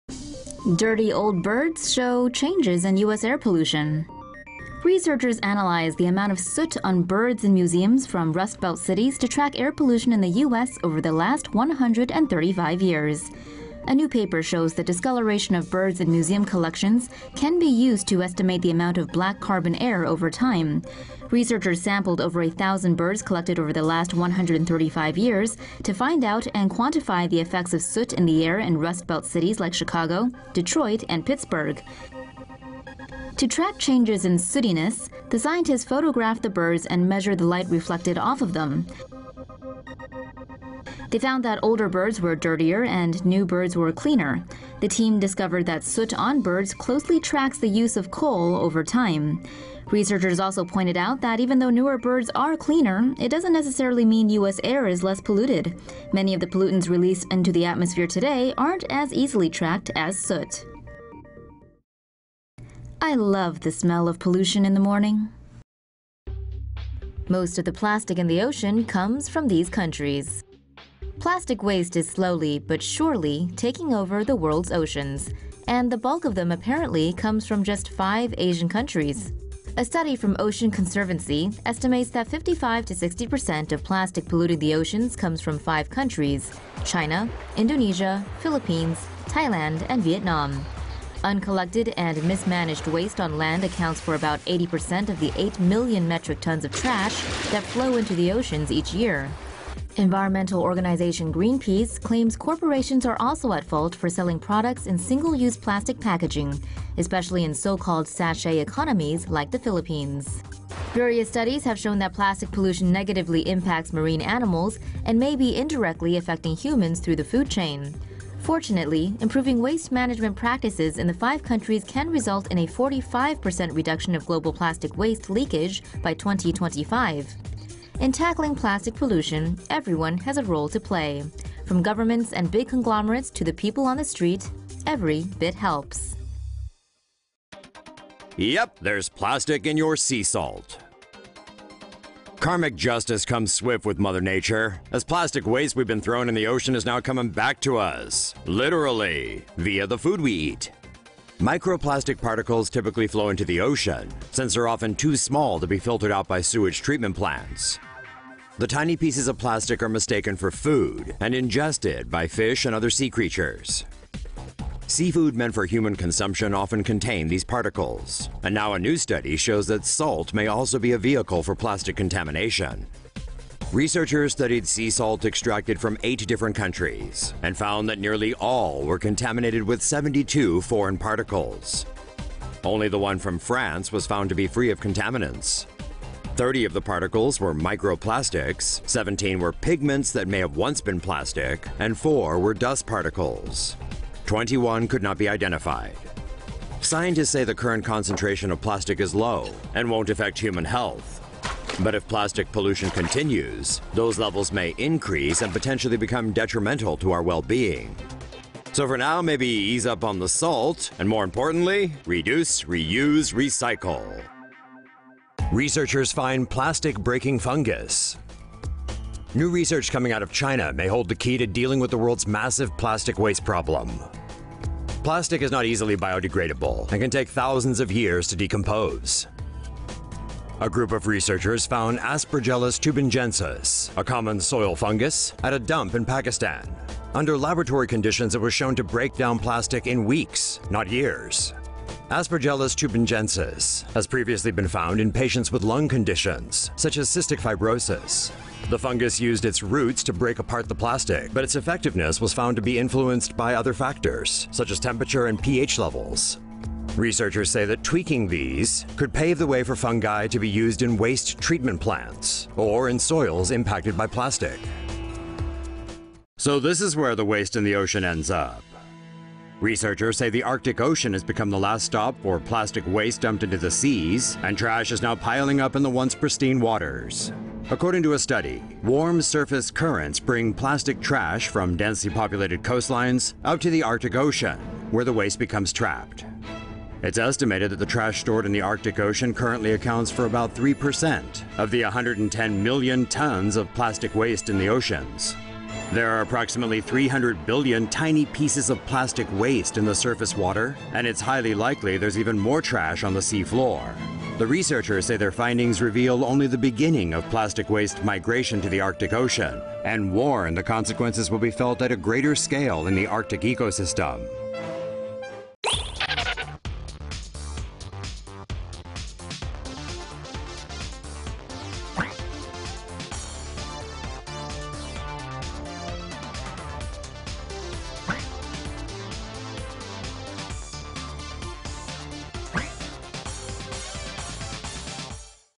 Air pollution: Dirty old birds can show changes in air pollution over last 135 years - TomoNews
CHICAGO — Researchers analyzed the amount of soot on birds in museums from Rust Belt cities to track air pollution over the last 135 years.

A new paper in the Proceedings of the National Academy of Sciences shows the discoloration of birds in museum collections can be used to estimate the amount of black carbon air over time, Science Daily reported.

Researchers from The Field Museum and the University of Chicago sampled over a thousand birds collected over the last 135 years to find out and quantify the effects of soot in the air in Rust Belt cities like Chicago, Detroit and Pittsburgh.

To track changes in sootiness, the scientists photographed the birds and measured the light reflected off of them.

They found that older birds were dirtier and new birds were cleaner. The team discovered that soot on birds closely tracks the use of coal over time.

Researchers also pointed out that even though newer birds are cleaner, it doesn't necessarily mean U.S. air is less polluted. Many of the pollutants released into the atmosphere today aren't as easily tracked as soot.